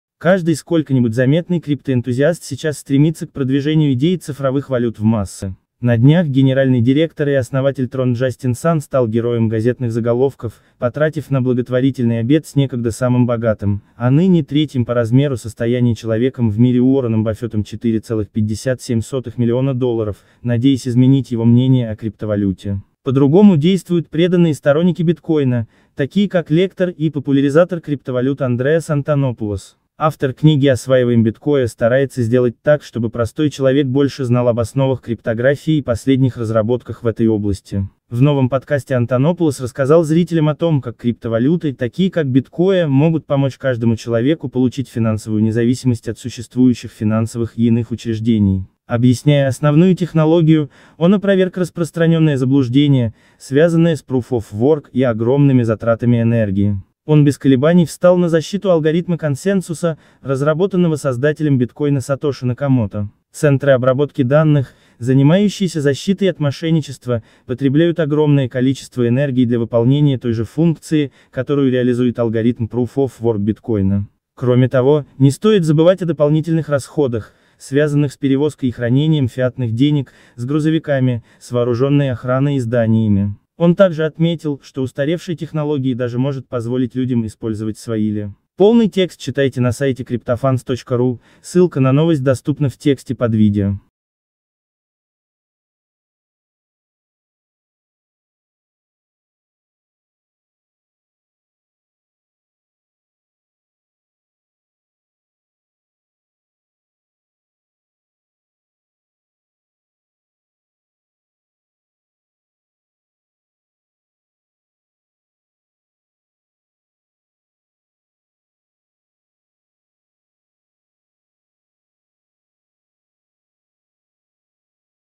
Андреас Антонопулос: банковские дата-центры потребляют не меньше энергии, чем proof-of-work
Каждый сколько-нибудь заметный криптоэнтузиаст сейчас стремится к продвижению идеи цифровых валют в массы. На днях генеральный дир